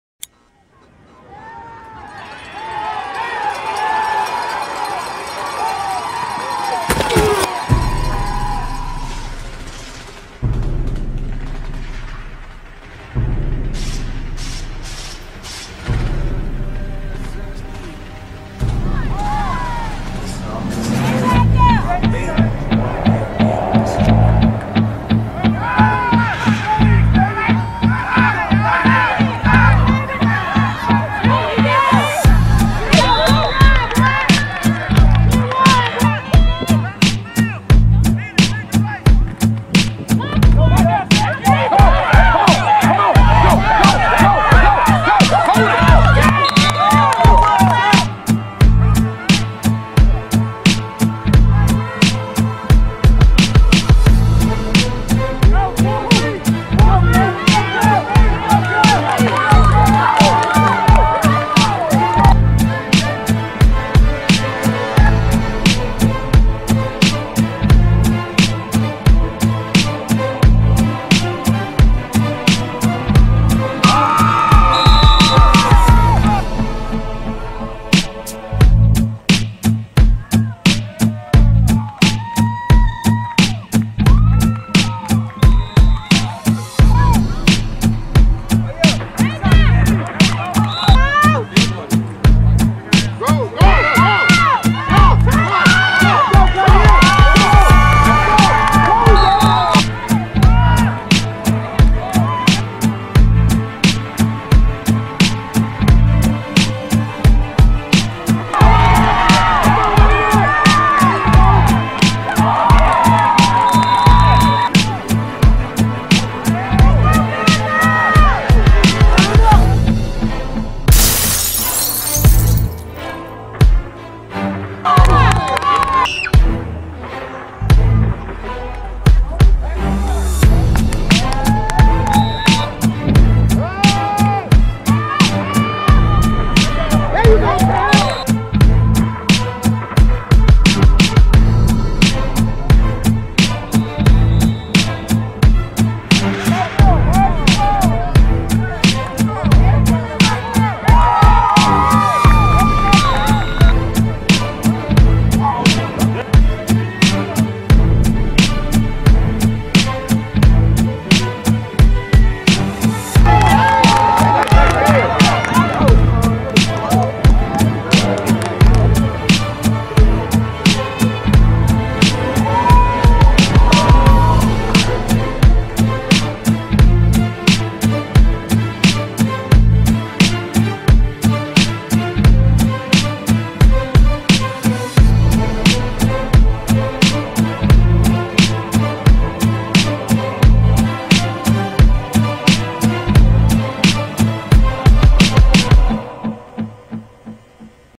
9U SHOAL CREEK vs PITTMAN 9 27 15
Shoal Creek Eagles vs Pittman Panthers
at Maynard Jackson High School
with D1spects Super Sunday Showcase